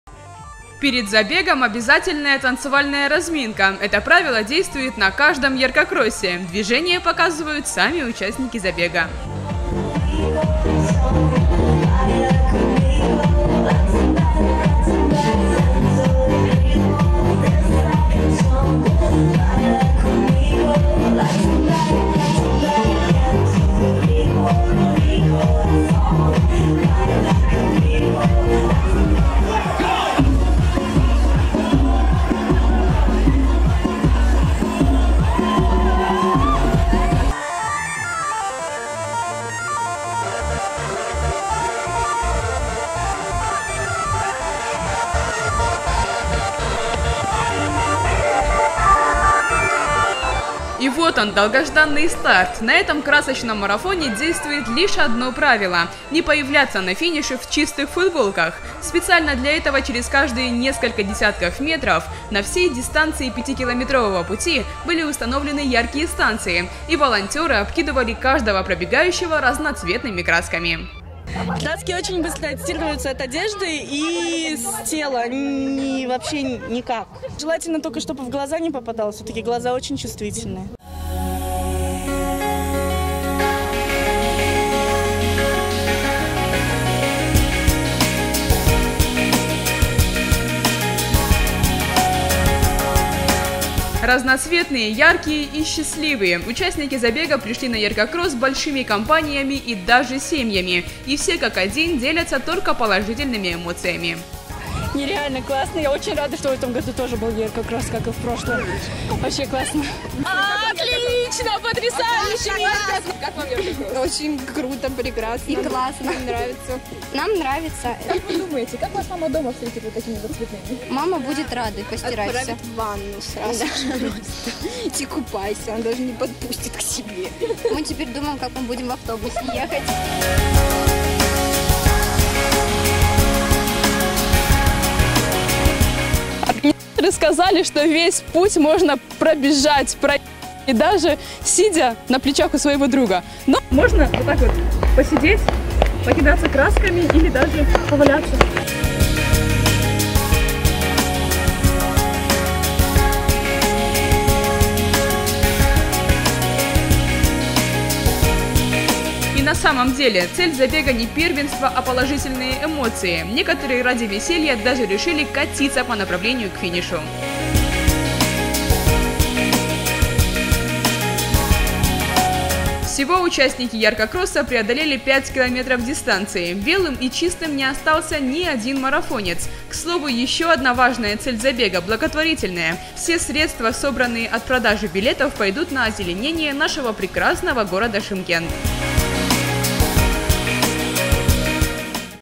В Шымкенте в третий раз прошёл Яркокросс
Сотни молодых людей города собрались в дендропарке Шымкента, чтобы принять участие в ярком марафоне - Яркокроссе. На всем пути 5-километрового забега участников закидывают разноцветными красками.